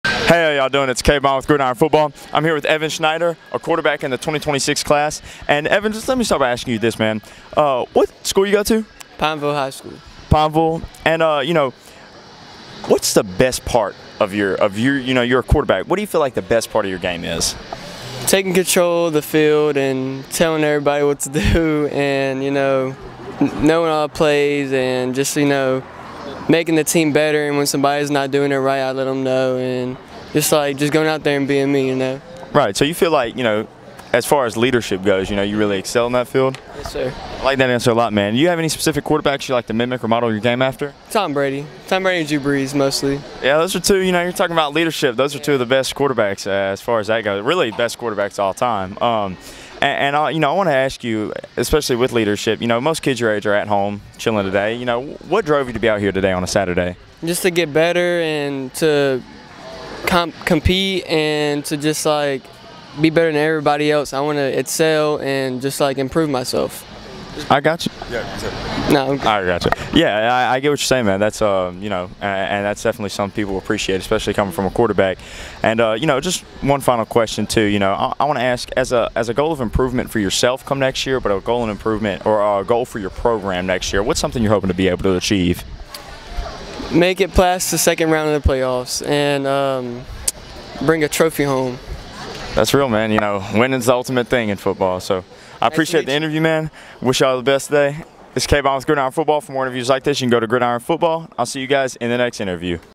Pineville High School QB Evan Schneider
Writer for Gridiron Football, Cabe Bond, interviews class of 2026 QB Evan Schneider at Gridiron's Elite Camp at East Ascension's Spartan Stadium. For more information on how to participate in our Elite Camps go